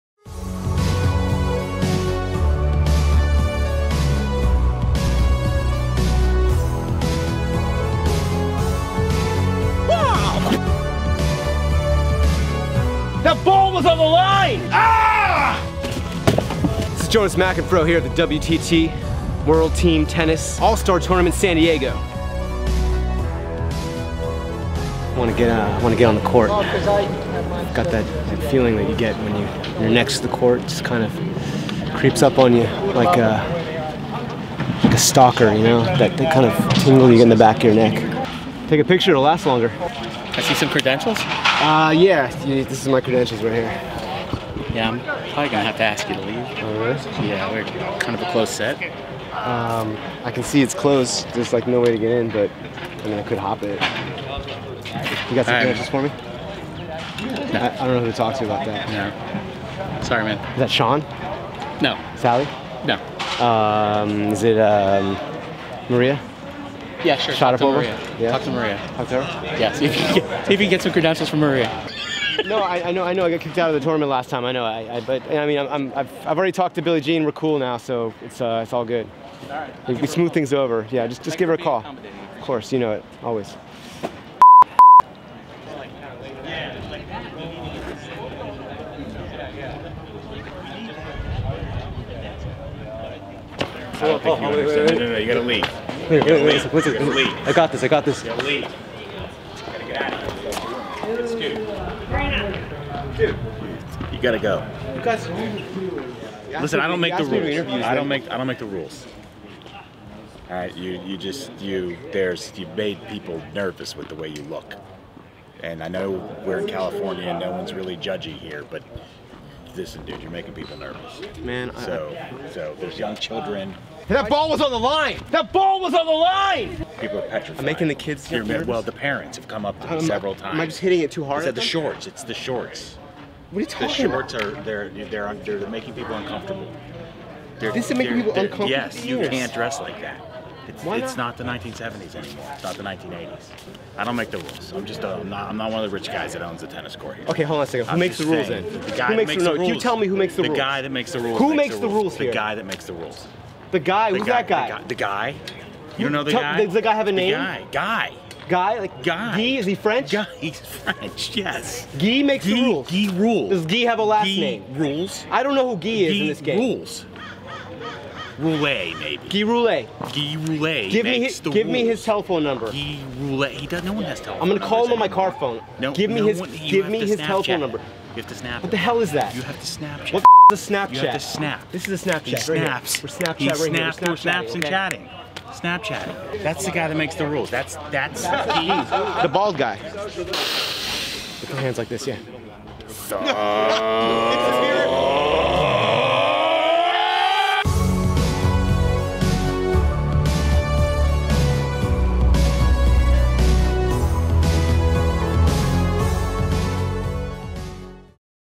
McEnfro at the 2020 WTT All-Star Match: "Who is in charge here?
WTT's McEnfro is looking for 'Guy Rule' and he is ready to ask some questions!